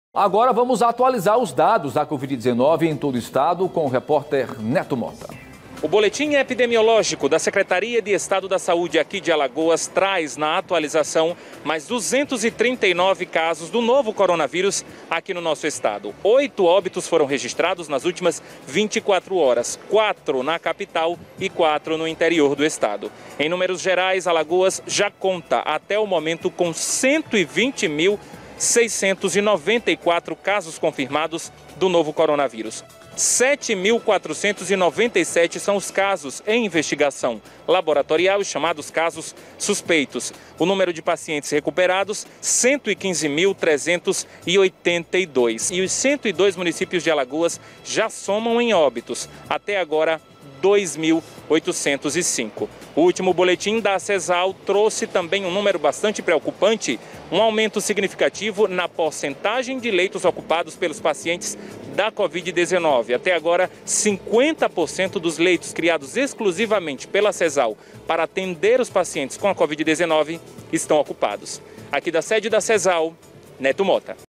Covid-19: Alagoas registra 120.694 casos confirmados e 2.805 óbitos
O repórter Netto Motta tem maus detalhes sobre os dados.